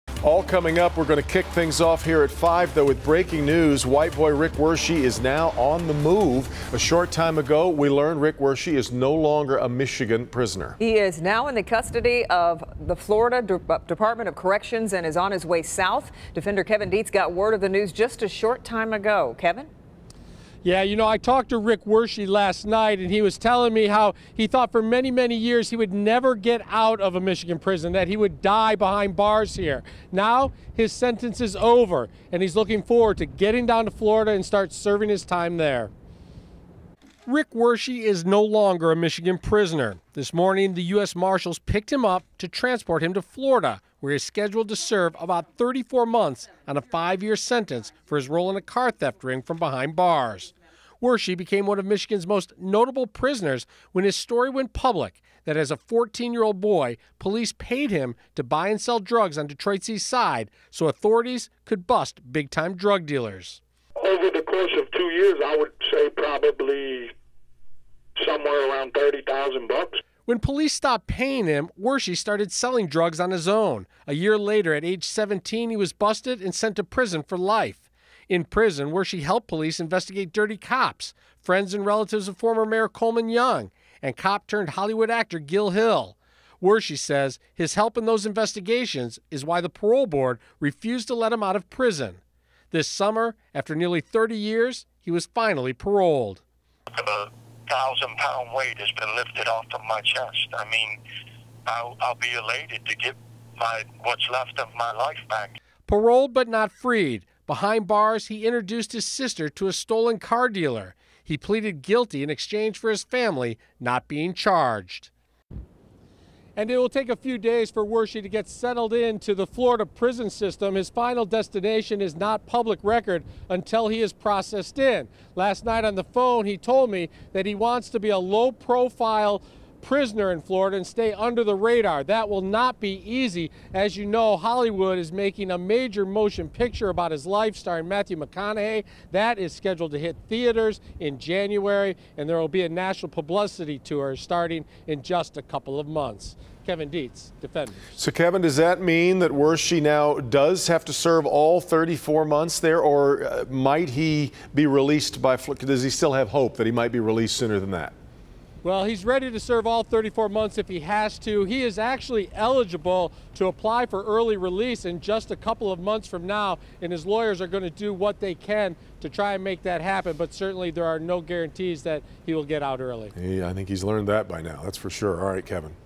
Rick Wershe to serve Florida sentence after being released from Michigan prison
For the first time in more than 29 years, "White Boy" Rick Wershe is no longer a Michigan prisoner. He's now in the custody of the Florida Department of Corrections and he's on his way south.